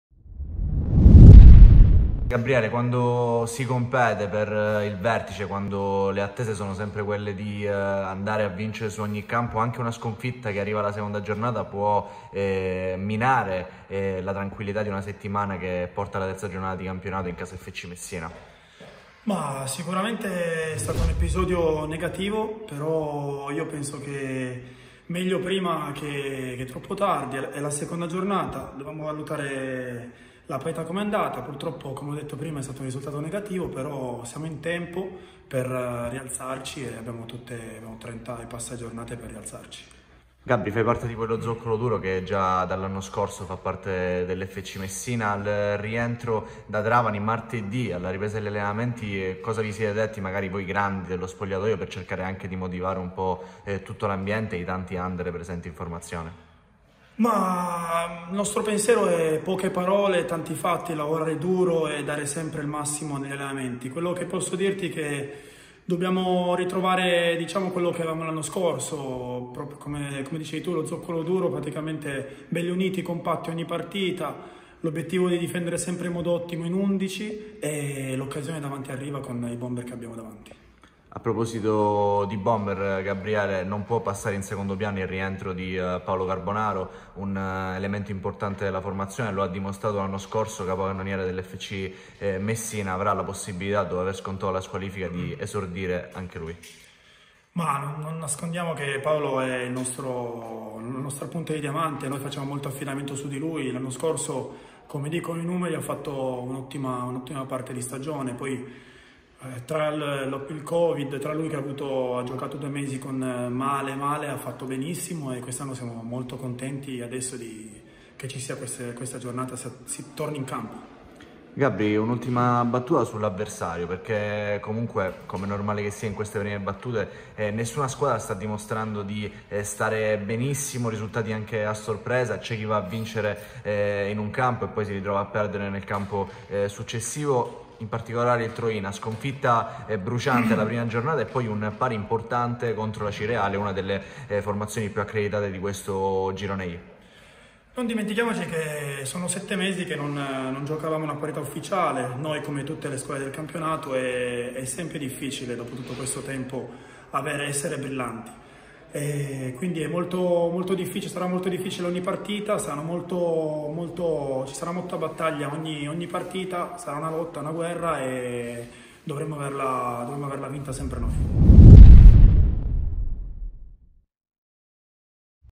Gabriele Quitadamo - FC Messina
Il difensore ex Cuneo, in sala stampa nella settimana che precede la sfida interna con il Troina.